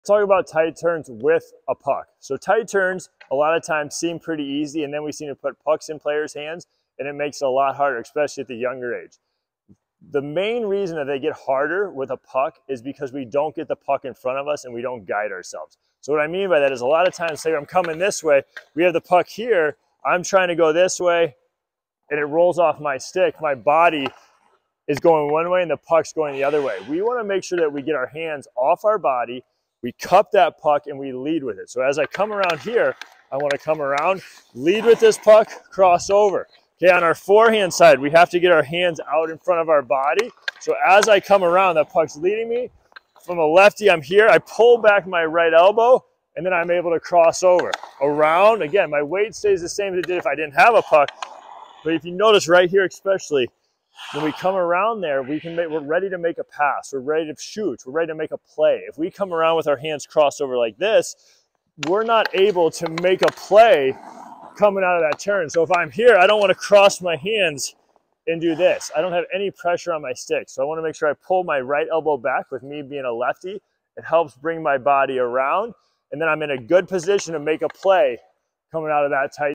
Elite Hockey Wednesdays: Episode 15: Tight Turns With A Puck
When making a tight turn make sure to keep your hands away from the body, let the puck lead and don't tuck your hands over. One reason now to tuck your hands is when you come out of the turn you don't have the ability to pass or shoot right away. You also lose strength on your stick to win the battle.